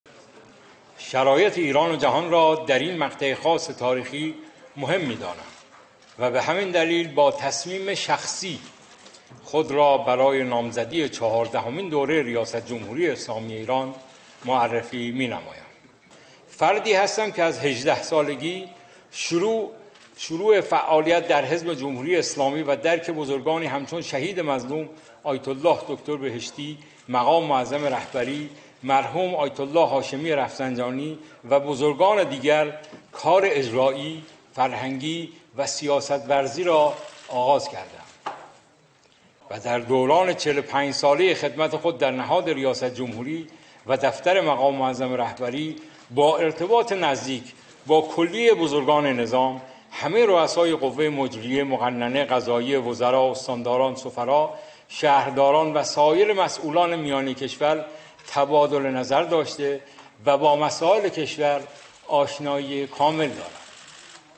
سخنرانی وحید حقانیان بعداز ثبت‌نام برای انتخابات ریاست جمهوری
وحید حقانیان مهره خامنه‌ای برای مهندسی انتخابات  امروز بعداز ثبت‌نام گذشته از یکسری محملات کلی فقط دو حرف واضح داشت:
• کاندید شدن من یک تصمیم شخصی بوده است. یعنی شما فکر نکنید که به دستور خامنه‌ای بوده.
• من از ۱۸ سالگی در مرکز قدرت بوده‌ام و دست همه شما برای من رو  است پس کنار بکشید.
بحران و جنگ و دعوای انتخابات زودرس بعداز مرگ رئیسی بتدریج به اوج می‌رسد. خامنه‌ای از انقاض نظامش کوتاه نمی‌آید